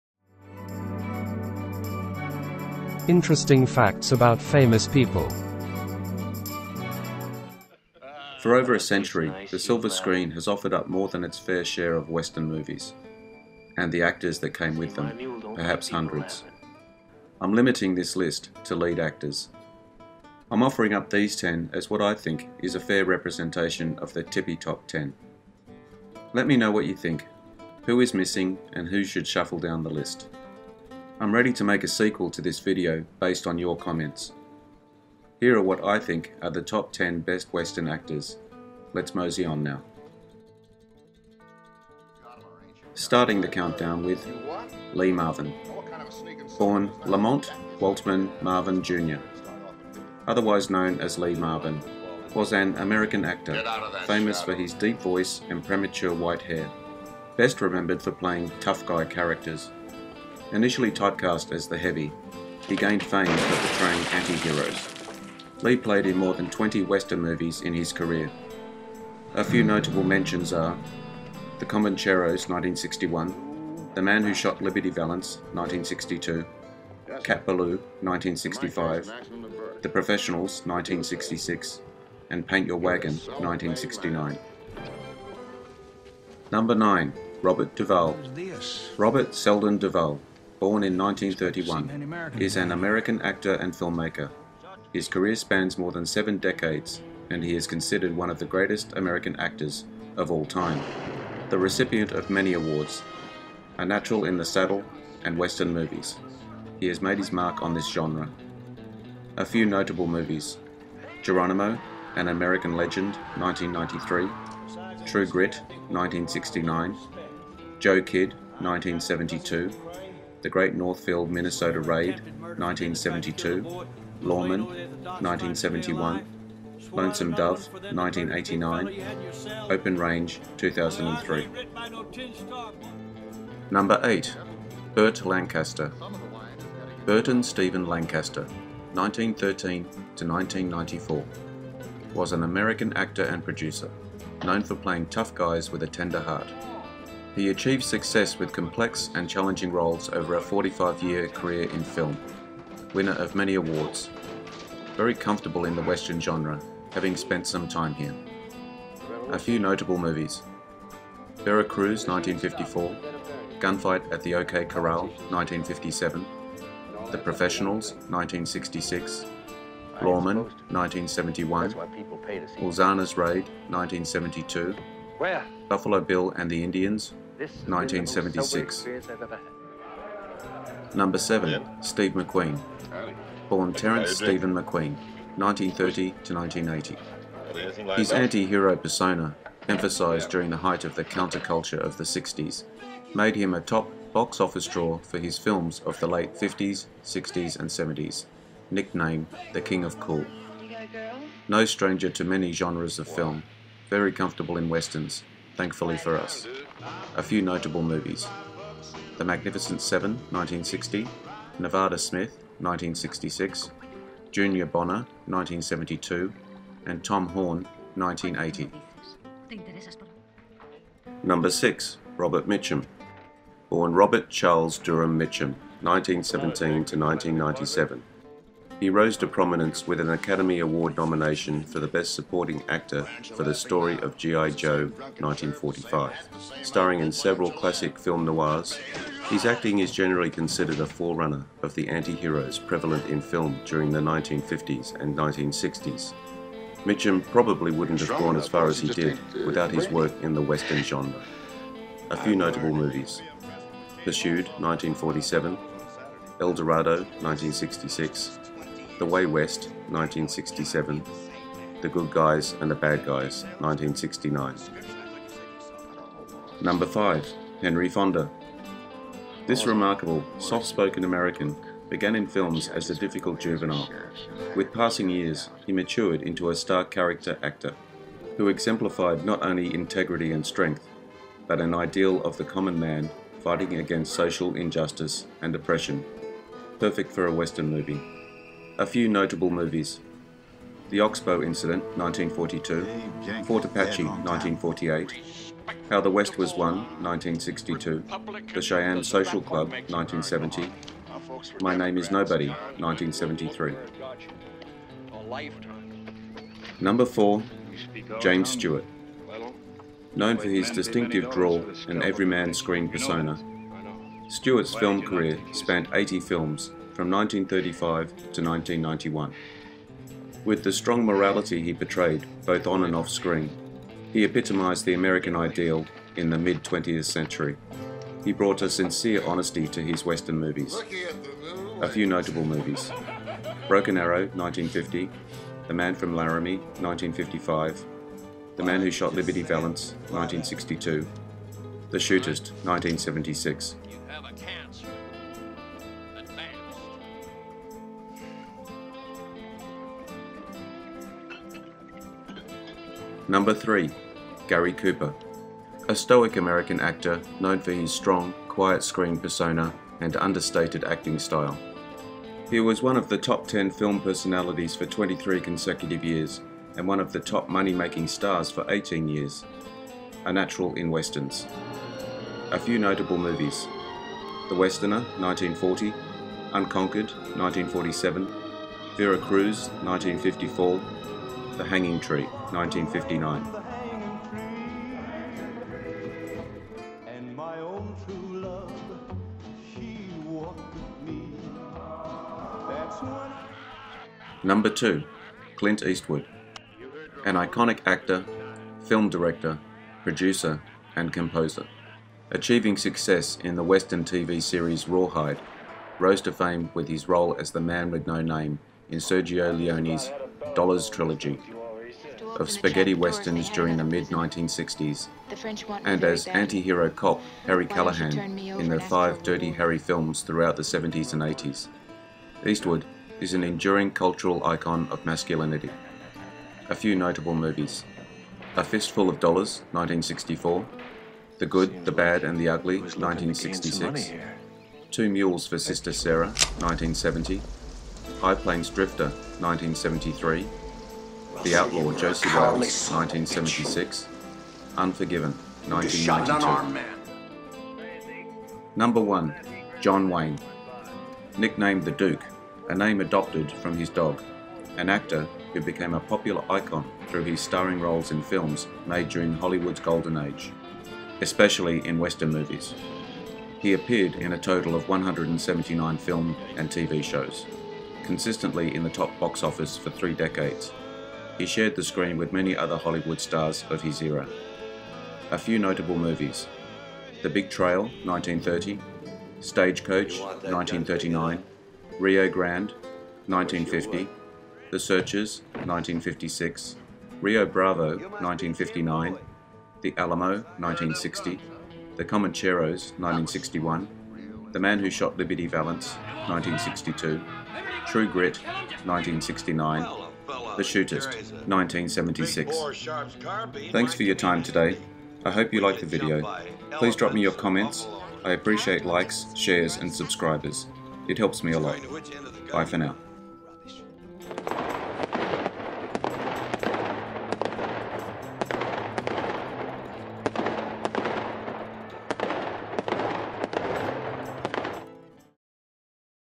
The Top 10 Western Actors of all times
The top 10 western actors of all time

For over a century, the silver screen has offered up more than its fair share of western movies. And the actors that came with them, perhaps hundreds. I’m limiting this list to lead actors.
I'm offering up these 10 as what I think is a fair representation of the tippy top 10.
Let me know what you think. Who is missing and who should shuffle down the list?
I’m ready to make a sequel to this video based on your comments.
Here are what I think are the top 10 best western actors, lets mozy on now…
Starting the countdown with…
10. Lee Marvin
Born Lamont Waltman Marvin Jr.; otherwise known as Lee Marvin, was an American actor. Famous for his deep voice and premature white hair, best remembered for playing "tough guy" characters. Initially typecast as the "heavy", he gained fame for portraying anti-heroes. Lee played in 25 western movies in his career.
9. Robert Duvall
Robert Selden Duvall, born in 1931, is an American actor and filmmaker. His career spans more than seven decades and he is considered one of the greatest American actors of all time. The recipient of many awards. A natural in the saddle and western movies. He has made his mark on this genre.
8. Burt Lancaster
Burton Stephen Lancaster (1913 – 1994) was an American actor and producer. Known for playing tough guys with a tender heart, he achieved success with complex and challenging roles over a 45-year career in film. Winner of many awards. Very comfortable in the western genre, having spent some time here.
7. Steve McQueen
Born Terrence Stephen McQueen (1930 – 1980). His antihero persona, emphasized during the height of the counterculture of the 60s, made him a top box-office draw for his films of the late 50s, 60s, and 70s. Nicknamed the "King of Cool".
No stranger to many genres of film. Very comfortable in westerns, thankfully for us.
6. Robert Mitchum
Born Robert Charles Durman Mitchum (1917 – 1997). He rose to prominence with an Academy Award nomination for the Best Supporting Actor for The Story of G.I. Joe (1945), starring in several classic film noirs. His acting is generally considered a forerunner of the antiheroes prevalent in film during the 1950s and 1960s. Mitchum probably wouldn't have gone as far as he did without his work in the western genre.
A few notable movies…
Pursued (1947)
El Dorado (1966)
The Way West (1967)
The Good Guys and the Bad Guys (1969)
5. Henry Fonda
This remarkable, soft-spoken American began in films as a difficult juvenile. With passing years, he matured into a star character actor who exemplified not only integrity and strength, but an ideal of the common man fighting against social injustice and oppression. Perfect for a Western movie.
The Ox-Bow Incident (1942)
Fort Apache (1948)
How the West Was Won (1962)
The Cheyenne Social Club (1970)
My Name Is Nobody (1973)
4. James Stewart
Known for his distinctive drawl and everyman screen persona, Stewart's film career spanned 80 films from 1935 to 1991. With the strong morality he portrayed both on and off screen, he epitomized the "American ideal" in the mid-twentieth century. He brought a sincere honesty to his western movies.
Broken Arrow (1950)
The Man from Laramie (1955)
The Man Who Shot Liberty Valance (1962)
The Shootist (1976)
3. Gary Cooper
An stoic American actor known for his strong, quiet screen persona and understated acting style. He was one of the top 10 film personalities for 23 consecutive years and one of the top money-making stars for 18 years. A natural in westerns.
The Westerner (1940)
Unconquered (1947)
Vera Cruz (1954)
The Hanging Tree (1959)
2. Clint Eastwood
An iconic actor, film director, producer and composer. Achieving success in the Western TV series Rawhide, rose to fame with his role as the "Man with No Name" in Sergio Leone's "Dollars Trilogy" of Spaghetti Westerns during the mid-1960s and as antihero cop Harry Callahan in the five Dirty Harry films throughout the 1970s and 1980s. Eastwood is an enduring cultural icon of masculinity.
A Fistful of Dollars (1964),
The Good, the Bad and the Ugly (1966),
Two Mules for Sister Sara (1970),
High Plains Drifter (1973),
The Outlaw Josey Wales (1976),
Unforgiven (1992).
1. John Wayne
Nicknamed The Duke, a name adopted from his dog, an actor who became a popular icon through his starring roles in films made during Hollywood's Golden Age, especially in Western movies. He appeared in a total of 179 film and TV shows. Consistently in the top box-office for three decades, he shared the screen with many other Hollywood stars of his era.
The Big Trail (1930)
Stagecoach (1939)
Rio Grande (1950)
The Searchers (1956)
Rio Bravo (1959)
The Alamo (1960)
The Comancheros (1961)
True Grit (1969)
The Shootist (1976)